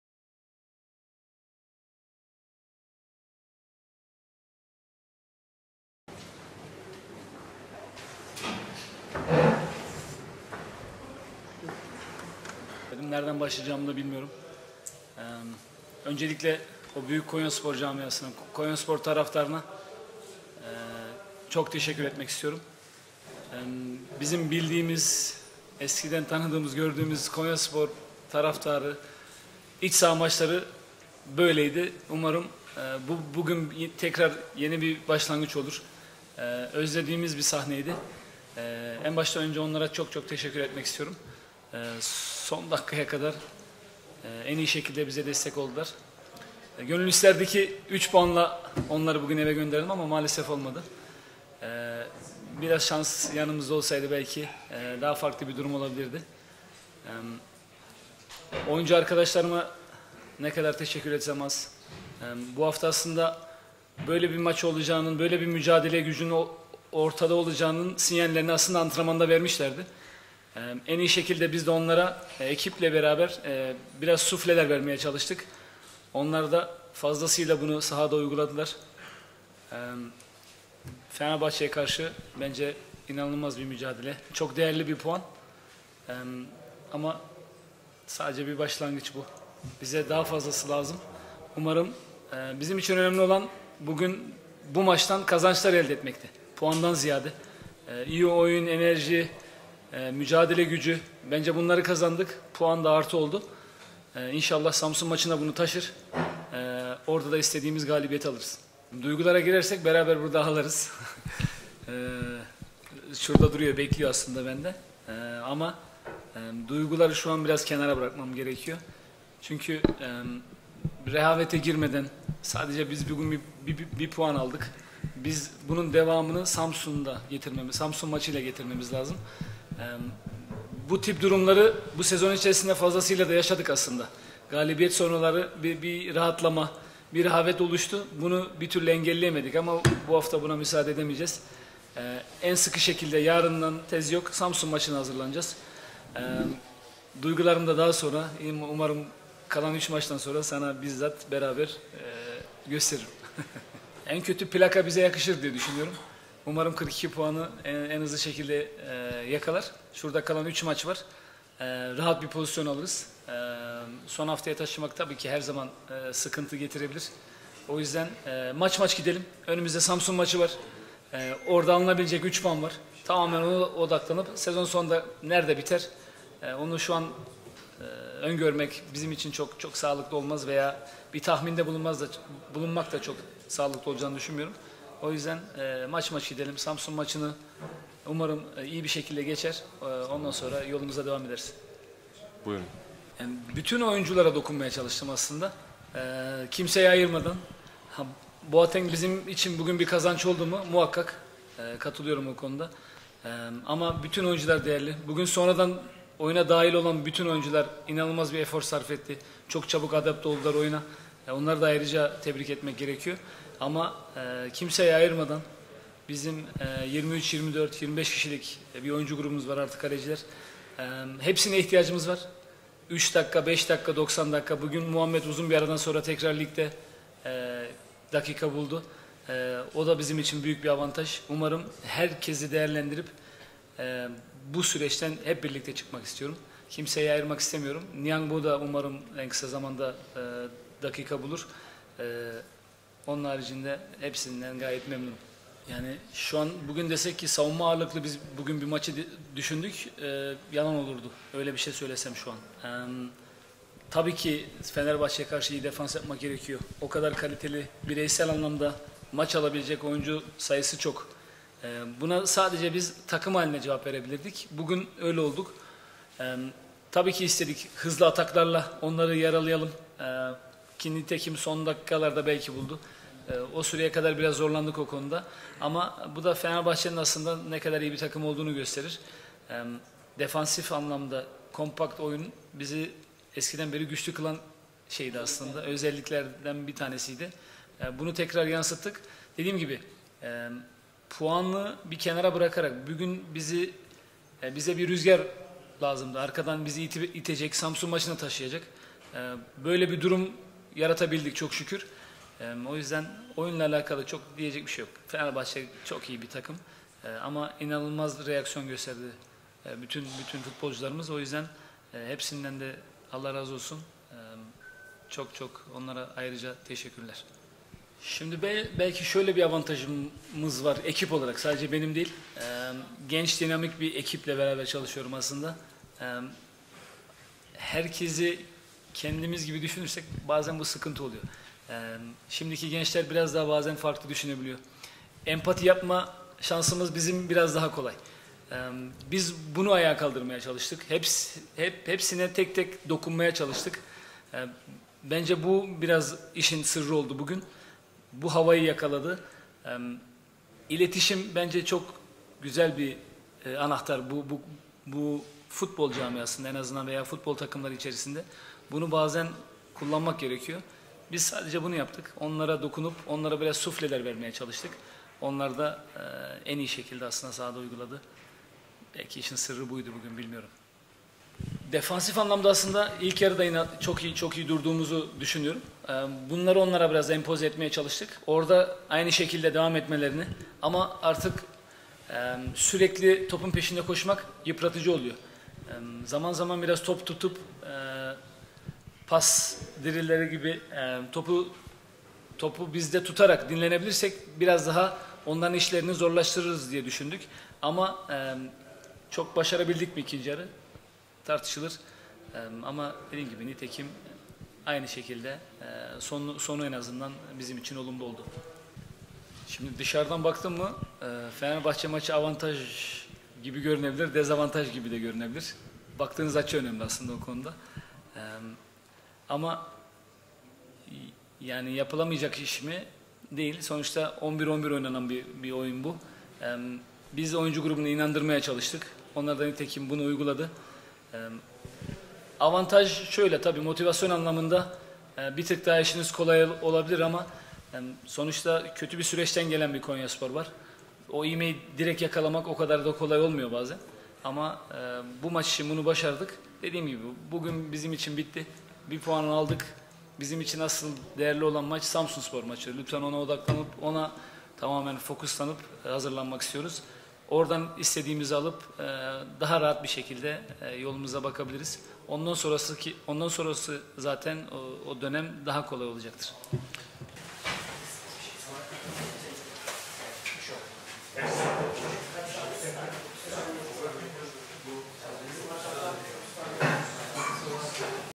Ali Çamdalı: "Fenerbahçe'ye karşı inanılmaz bir mücadele verdik"
TÜMOSAN Konyaspor Teknik Direktörü Ali Çamdalı: "Fenerbahçe'ye karşı inanılmaz bir mücadele verdik, çok değerli bir puan aldık"

Trendyol Süper Lig'in 35. haftasında sahasında Fenerbahçe ile 0-0 berabere kalan TÜMOSAN Konyaspor'da teknik direktör Ali Çamdalı, Fenerbahçe'ye karşı iyi mücadele verdiklerini, çok değerli bir puan aldıklarını söyledi.

Çamdalı, maçın ardından düzenlenen basın toplantısında, sahalarında aldıkları 1 puandan dolayı mutlu olduklarını dile getirdi.

Taraftarın desteğine teşekkür eden Çamdalı, şöyle konuştu:
"Umarım bugün tekrar yeni bir başlangıç olur. Özlediğimiz bir sahneydi. Gönül isterdi ki 3 puanla onları eve gönderelim ama maalesef olmadı. Biraz şans yanımızda olsaydı belki daha farklı bir durum olabilirdi. Oyuncu arkadaşlarıma ne kadar teşekkür etsem az. Böyle bir maç olacağının, böyle bir mücadele gücünü ortaya koyacaklarının sinyallerini aslında antrenmanda vermişlerdi. Biz de ekiple maçta sufleler vermeye çalıştık. Onlar da fazlasıyla bunu sahada uyguladılar. Fenerbahçe'ye karşı inanılmaz bir mücadele verdik, çok değerli bir puan aldık. Bu bir başlangıç. Bize daha fazlası lazım. Puandan ziyade iyi oyun, enerji ve mücadele gücü, bunları kazandık. Puan da artı oldu. İnşallah Samsunspor maçına bunu taşır, orada da istediğimizi alırız."